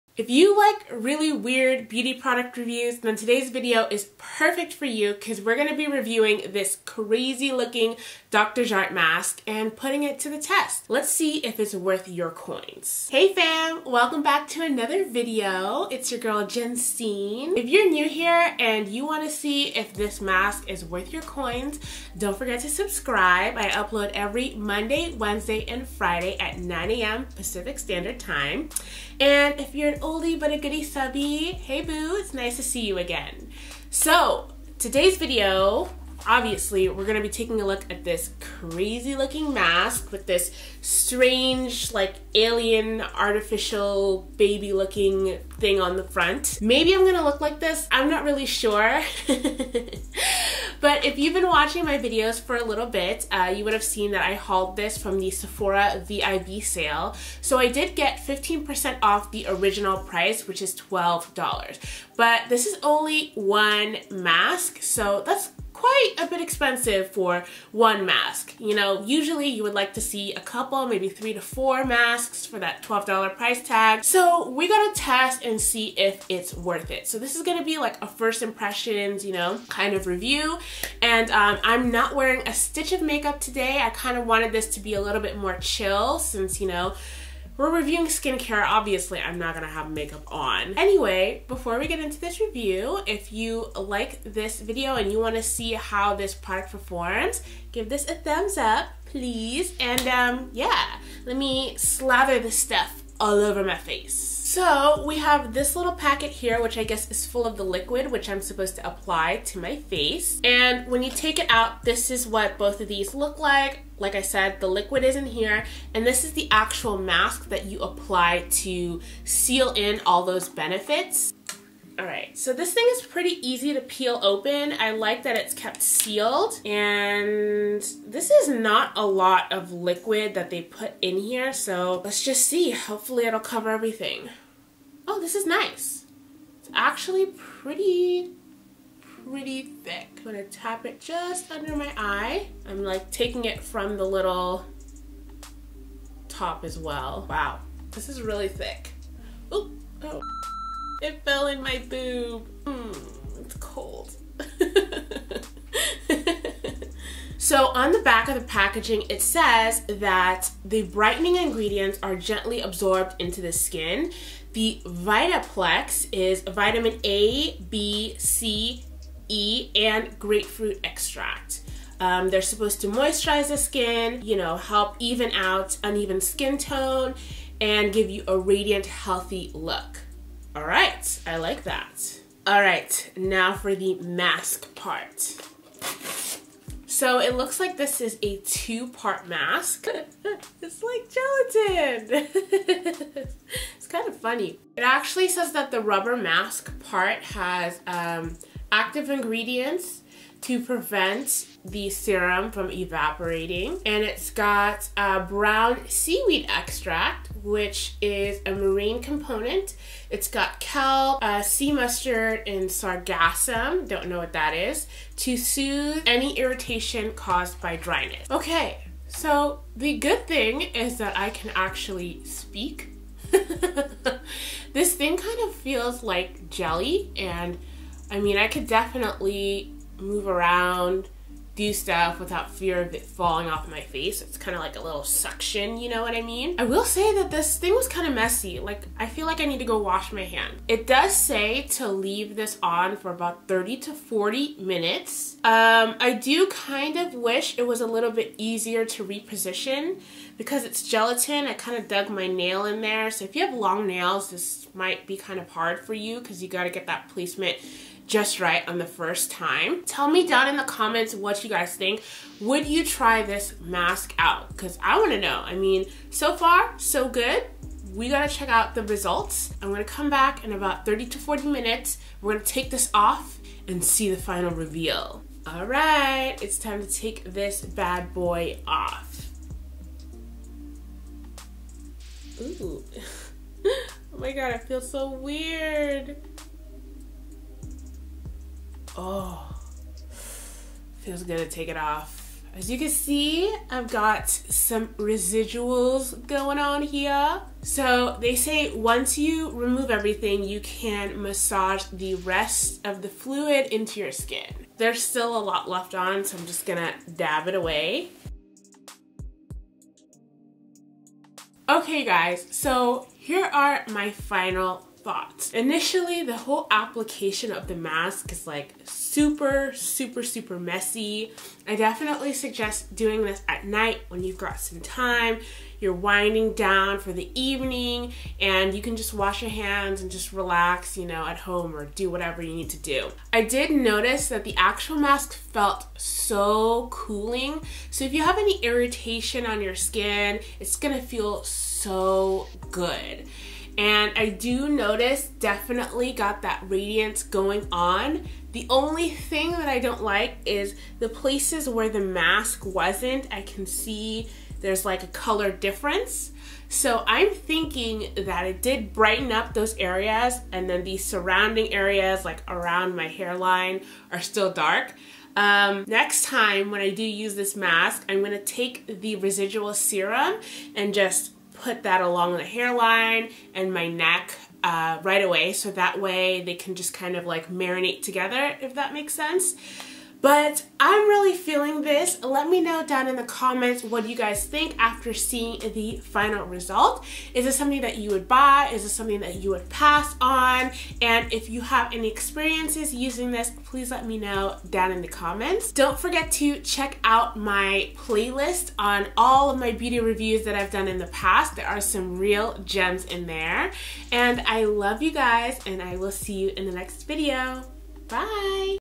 TRYING OUT THE NEW DR JART BRIGHT LOVER RUBBER MASK
The bright lover rubber mask claims to visibly brighten the look of uneven skin tone. The package contains an intensive brightening ampoule pack (brightening serum) and the brightening wrapping rubber mask. Oh and it is a single use mask. Would you try this?

→ DR JART BRIGHT LOVER RUBBER MASK $12

No Makeup.. Ha! 🤣
And an old scarf that I’ve had since college

WRITE TO ME HERE
Jensine Finn
34 Blair Park Rd